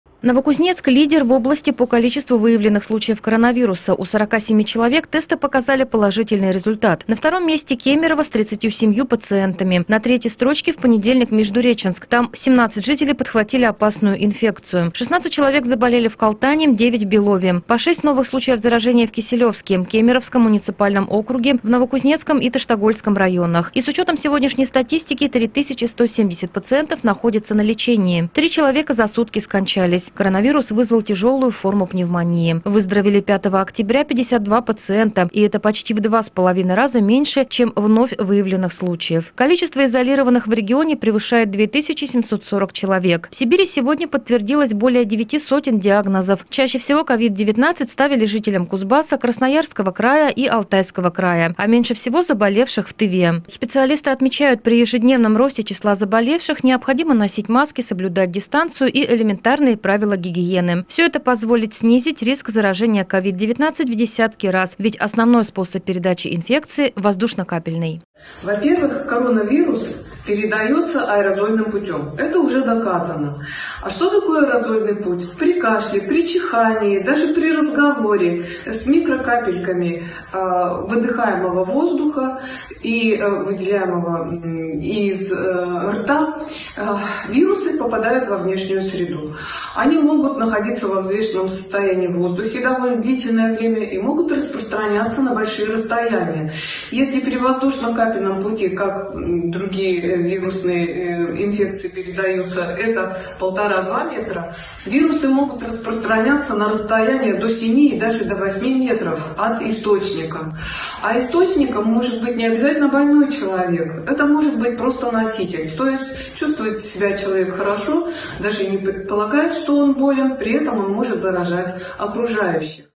5 октября: сводка по COVID-19
175 кузбассовцев заразились коронавирусом за сутки. Очередной максимальный показатель для региона. Выписанных в 2,5 раза меньше, чем выявленных. Ситуация по городам – в нашем обзоре.